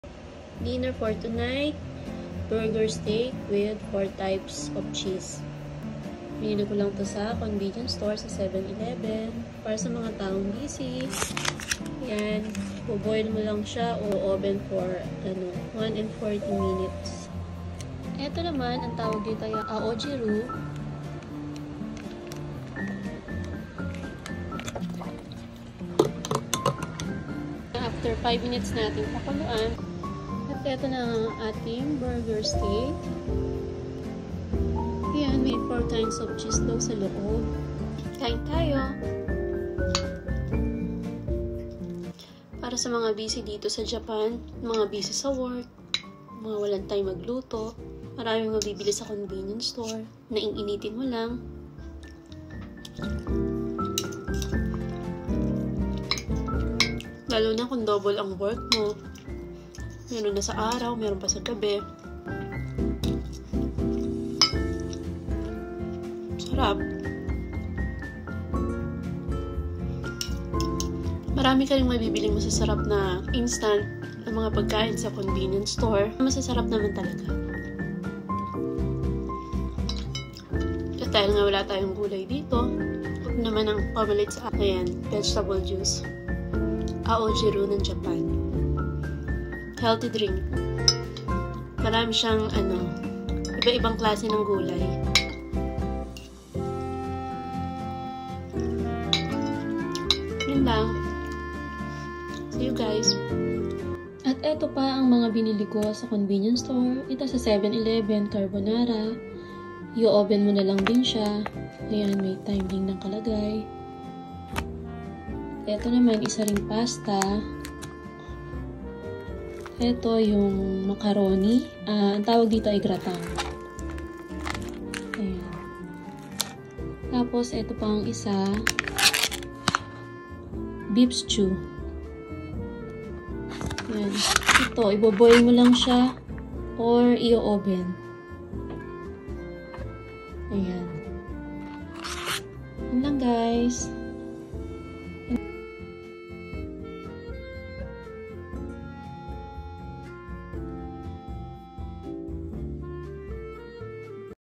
Living in Japan / Convenience Store Foods | KikaySandy in Japan
Easy to prepare, less consuming time
But you eat like a food in a Restaurant